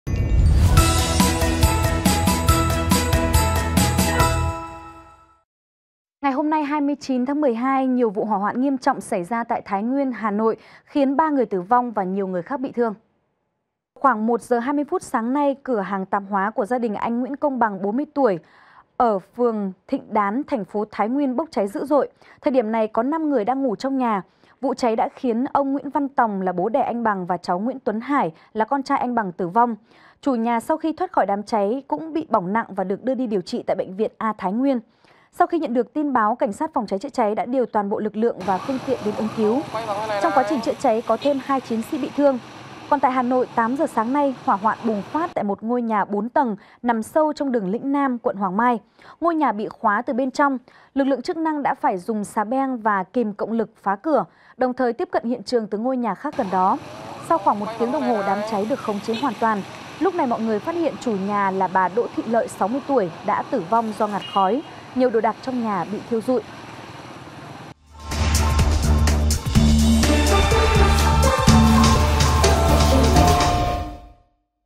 VTC Tin Mới
VTC | Vụ hỏa hoạn nghiêm trọng ở Thái Nguyên, Hà Nội đã gây ra hậu quả nghiêm trọng, 3 người tử vong.